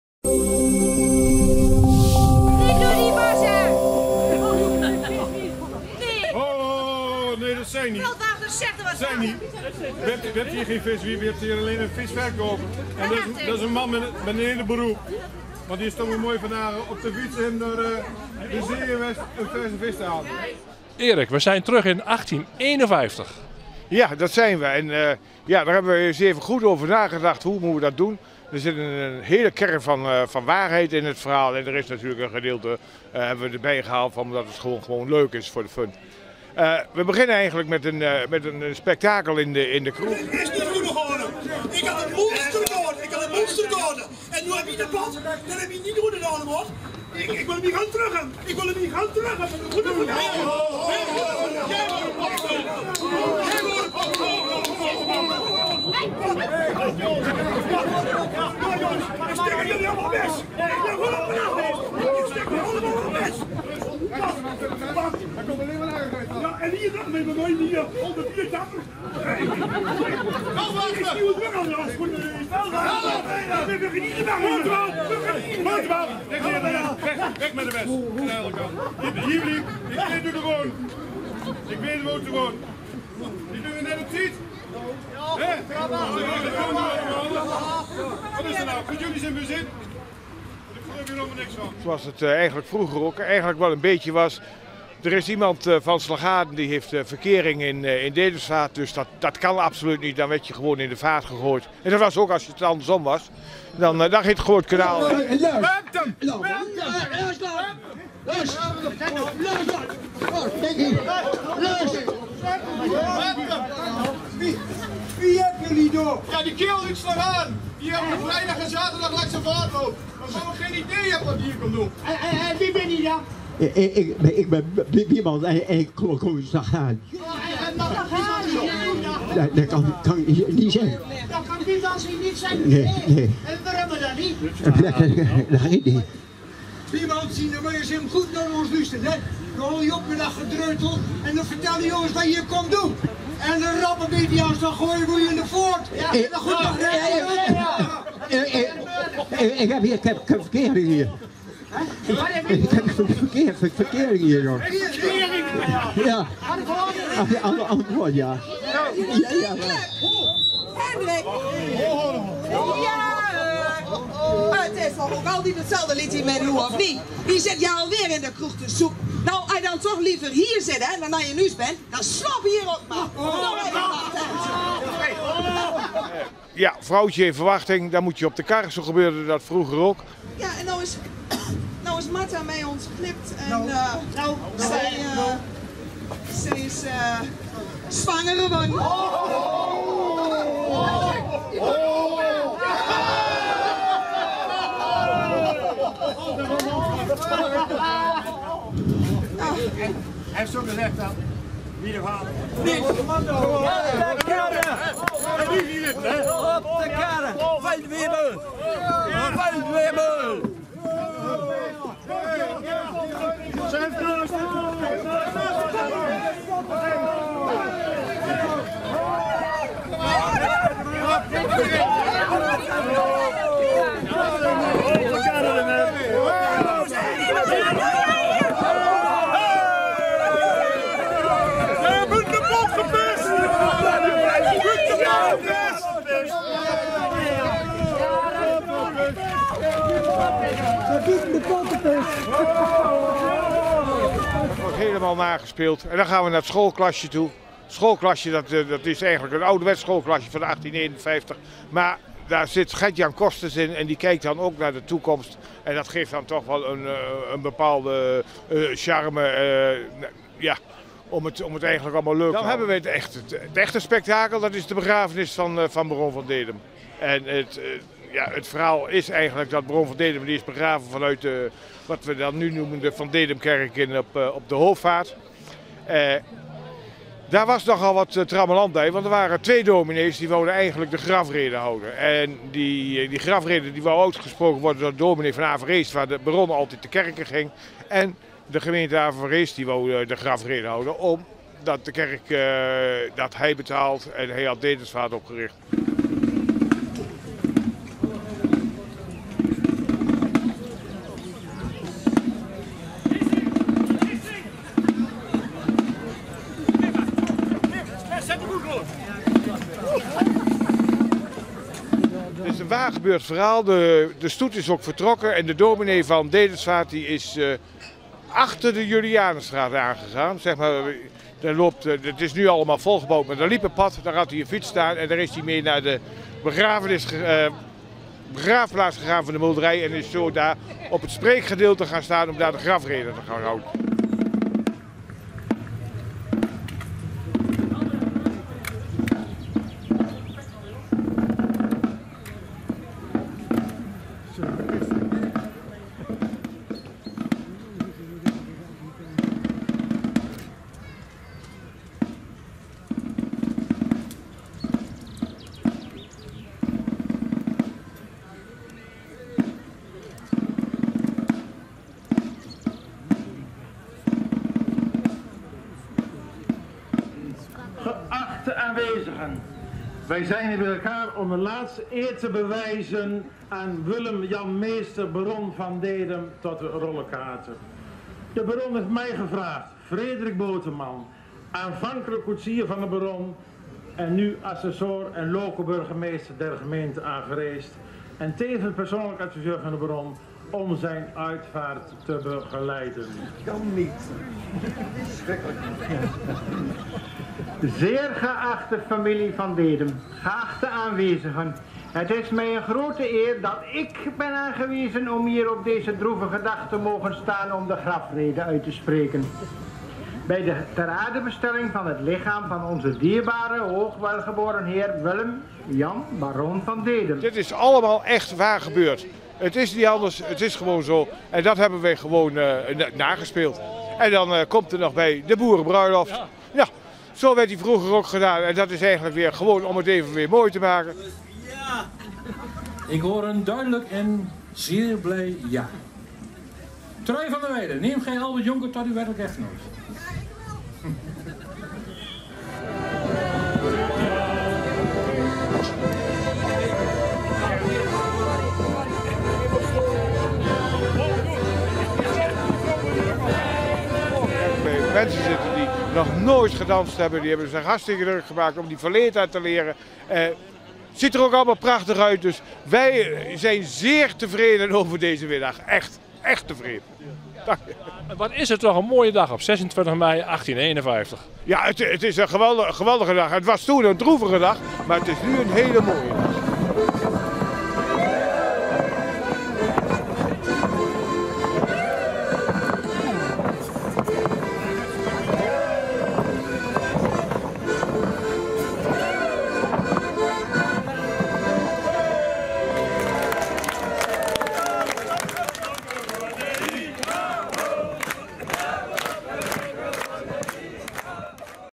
Dedemsvaart: Avereest historisch spektakel 2018
Een impressie van even terug naar 1851: Avereest huilt, lacht, rouwt en feest.